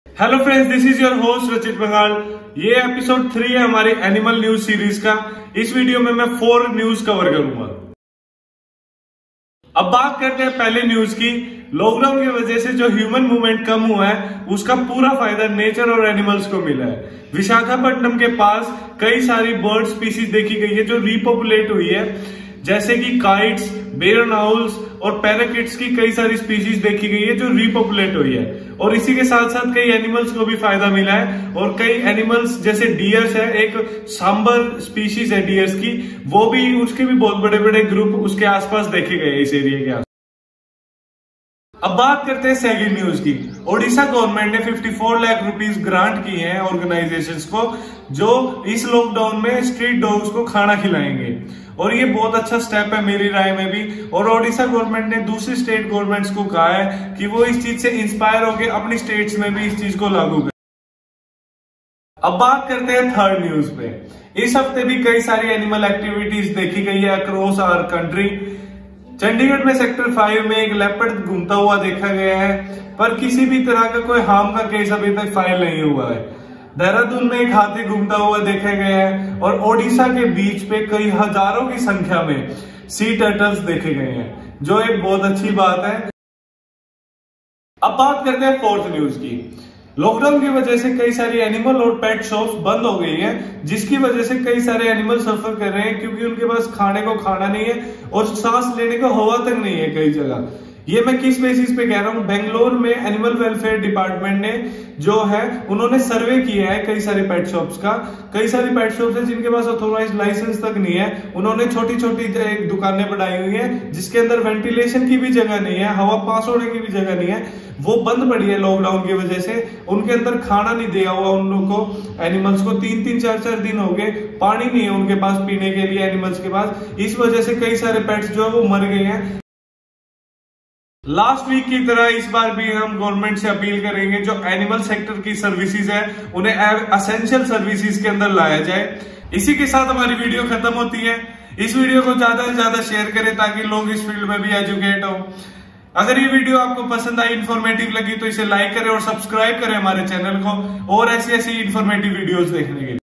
Animal news | Due to Lockdown Animals Repopulating in various parts of Country | Pet store abuse
IN THIS VIDEO ON ONE PLATFORM WE WILL COVER NEWS AND CURRENT AFFAIRS IN THE FEILD OF ANIMALS
WE HAVE COVERED 4  NEWS IN THIS EPISODE -3
GIVE US YOUR VALUABLE FEEDBACK ABOUT THIS VIDEO